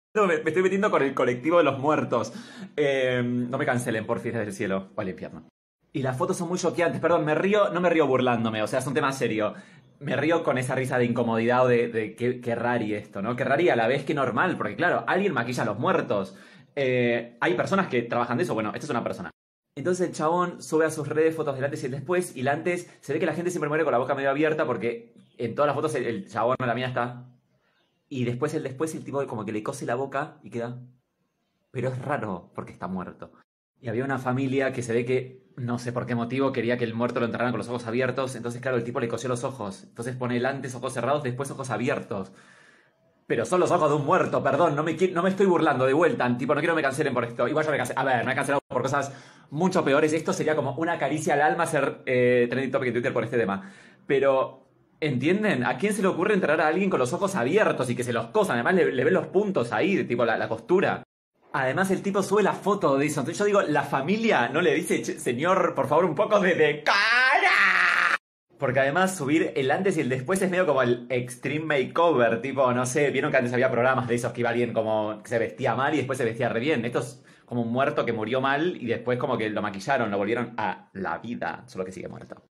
Martín Cirio, otra vez polémico: en plena pandemia, ahora se mete con los muertos
El influencer contó un hecho peculiar que vivió y que involucran a personas difuntas.